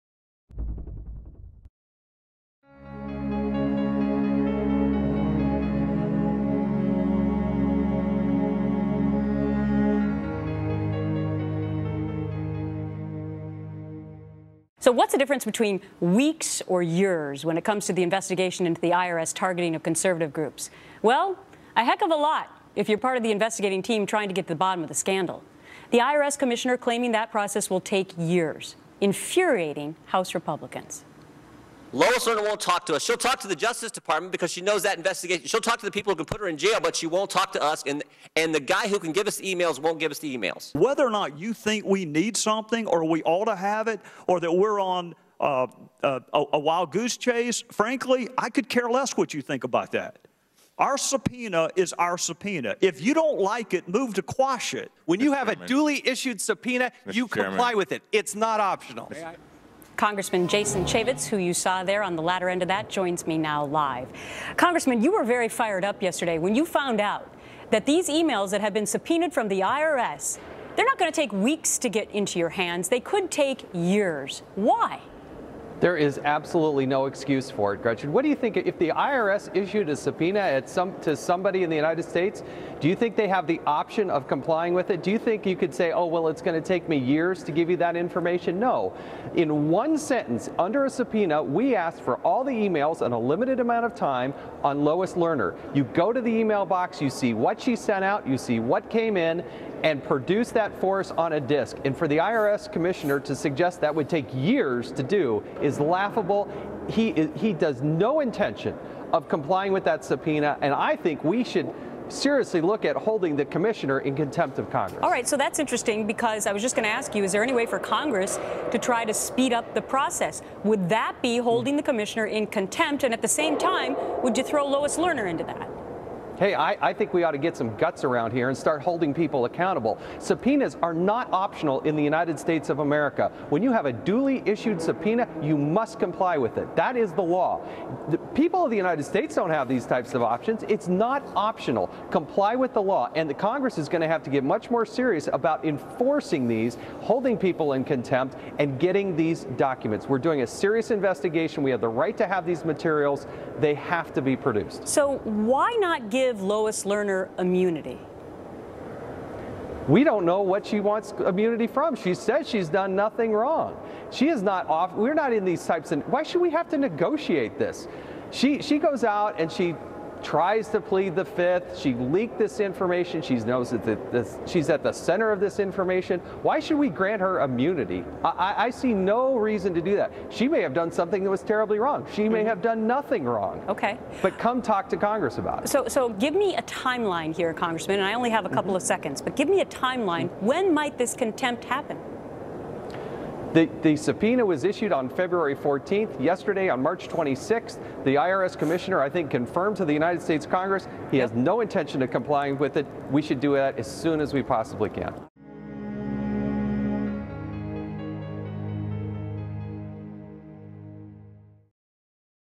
FOX NEWS: IRS Contempt For Subpoena
Congressman Jason Chaffetz (R-UT) discusses IRS stonewalling tactics and what can be done.